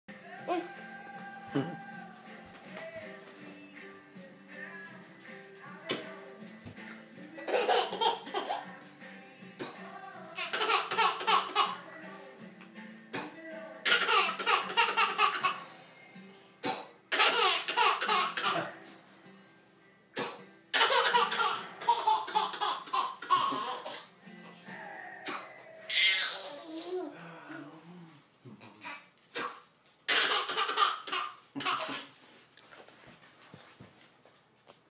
Making baby laugh
Apparently popping a baby bottle cap out of my mouth and catching it is hilarious...